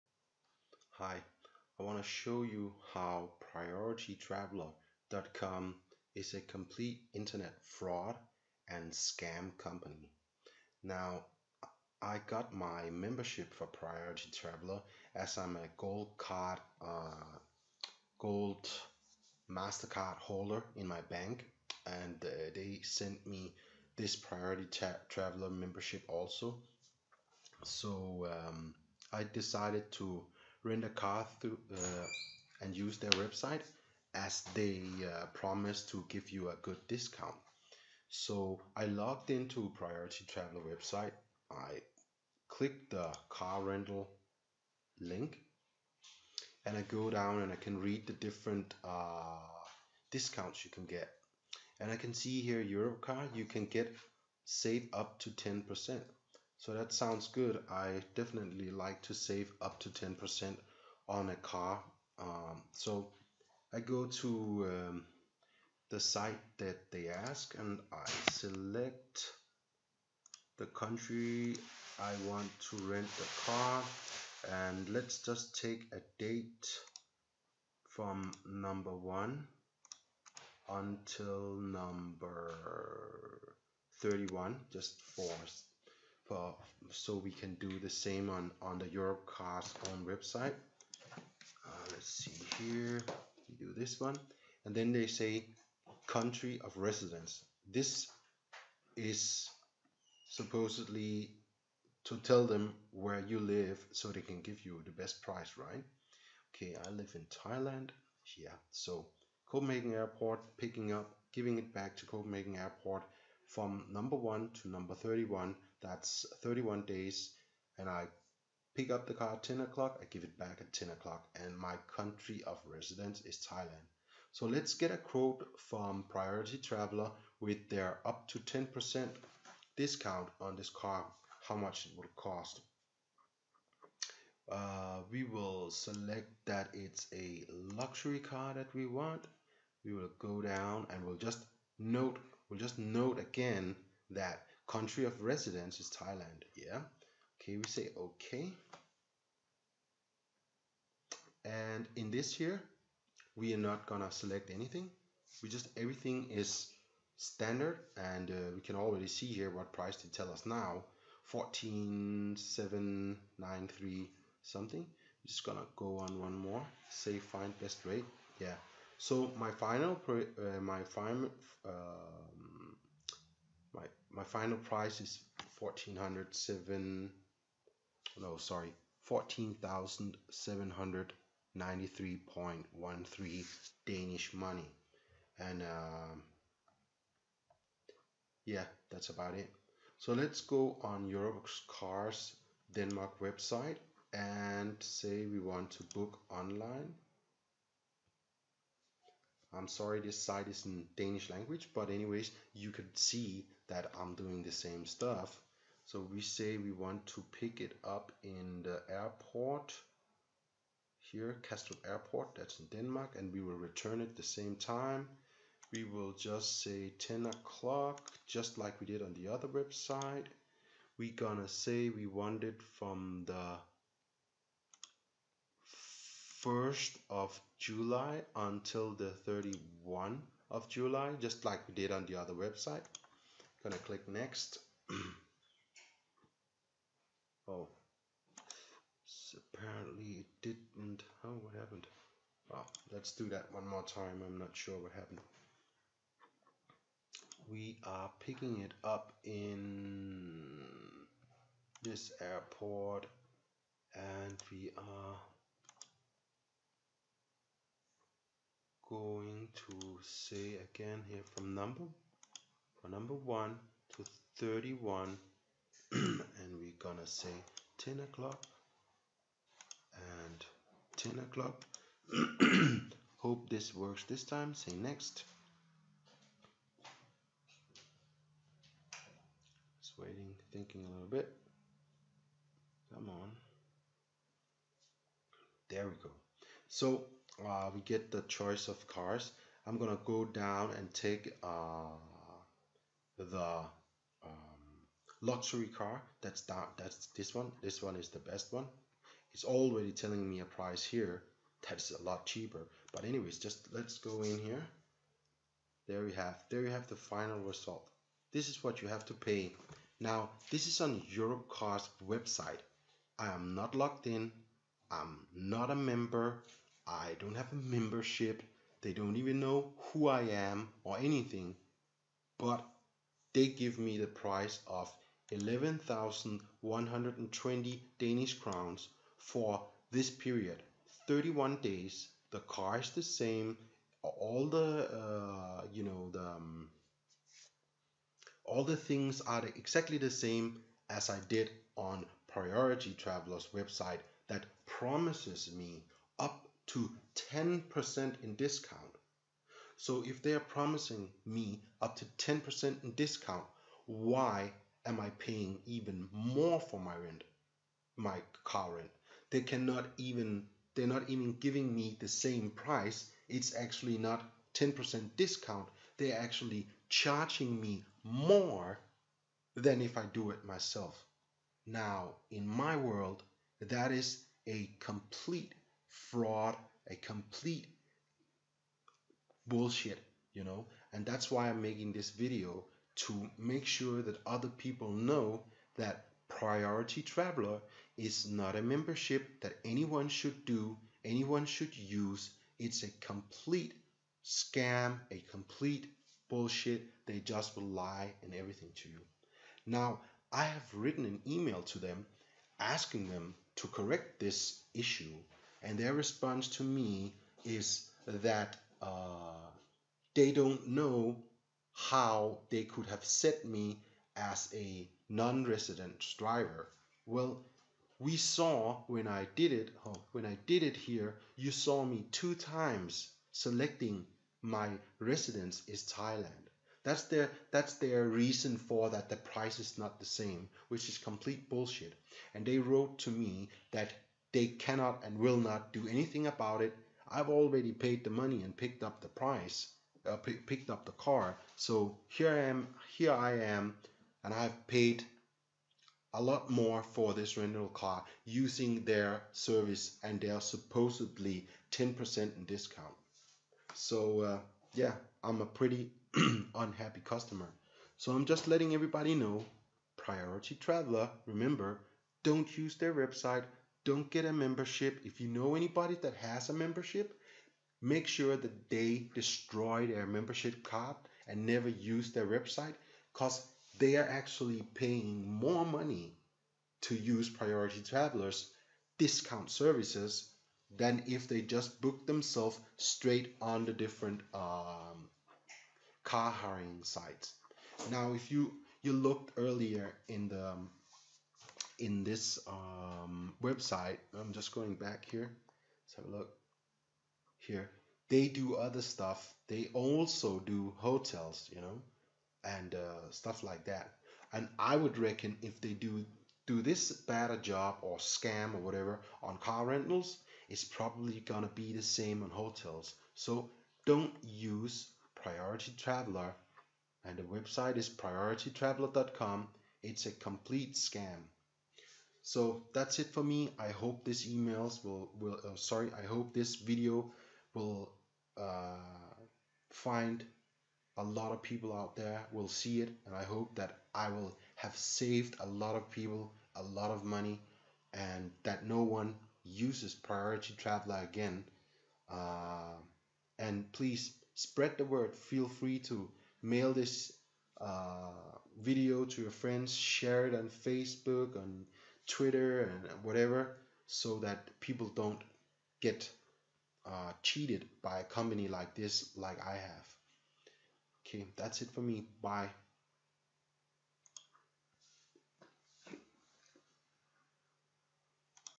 Priority Traveller - Prioritytraveller online scam Internet fraud!
Do not use "Priority traveller" website or there cards, it's a complete scam. They will cheat you out of more money then you have to pay, see my video for proof. I emailed them, and told them and explained, they just stopped answering me. Don't let companies like this stay in business when all they do is cheat you, lie, and take more money from you then you need to pay, there is no savings here! Only loss! Don't use Warning to all!

Please note "Europe Car" has nothing to do with this!